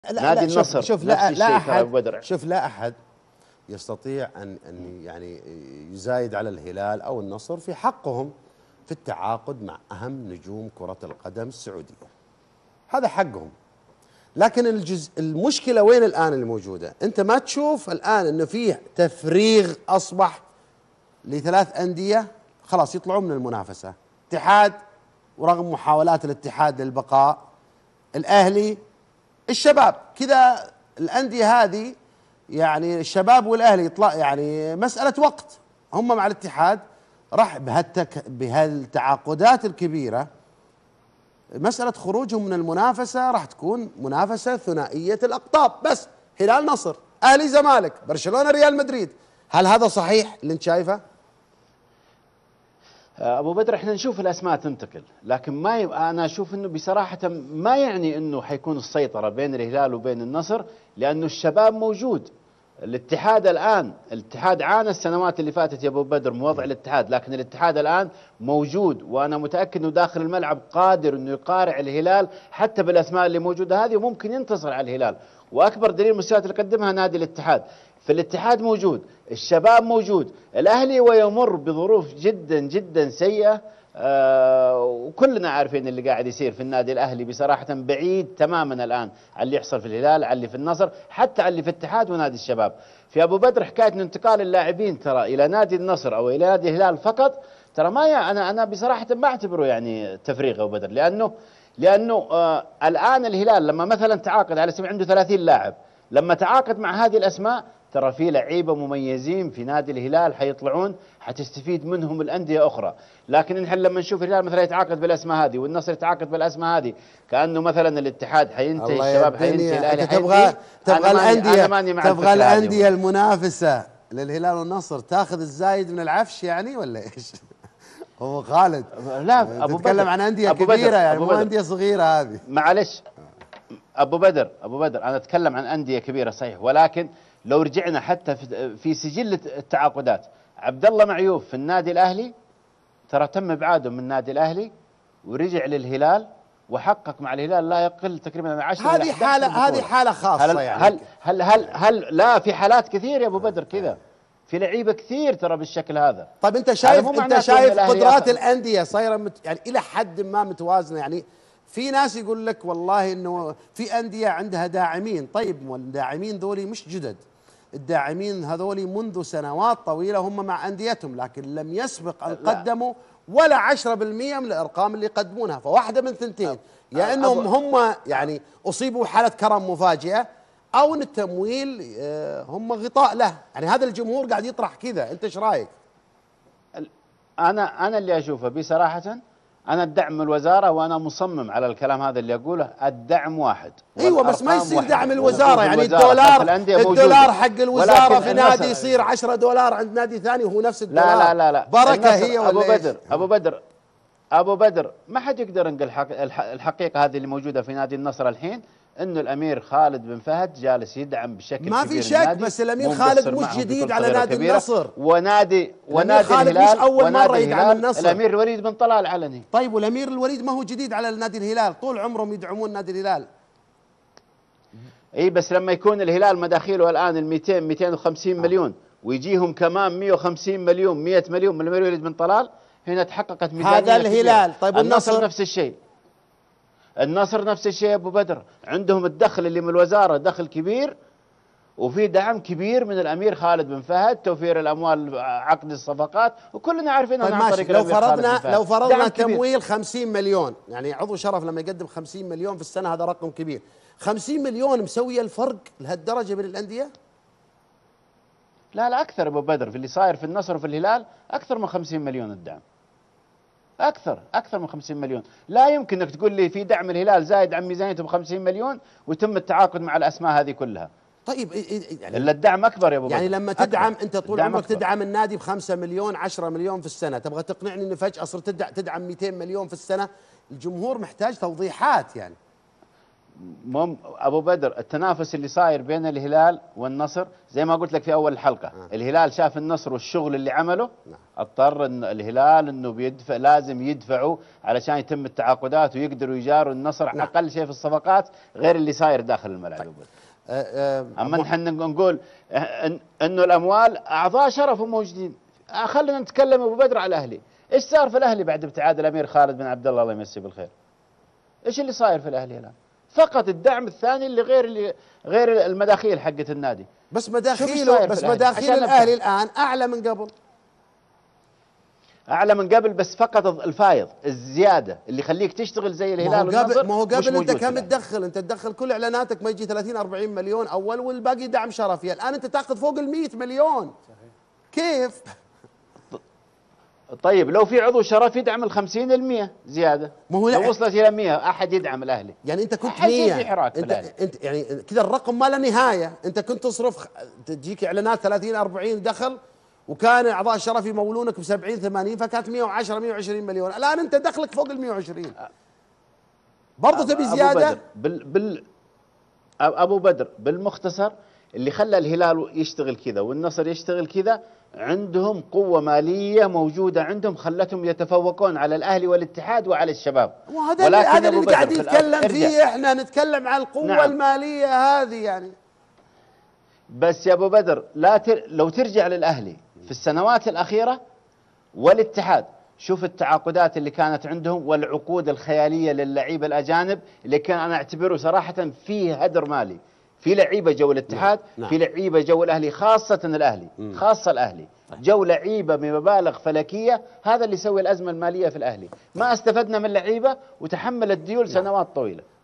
وليد_الفراج: هل ما يحدث في الهلال و النصر هو تفريغ من المنافسة؟
غرم العمري: الشباب و الاتحاد موجودان بالمنافسة.. توجد قوة مالية في الهلال و النصر تصنع الفرق والدعم يصل لأكثر من 50 مليون سنويًا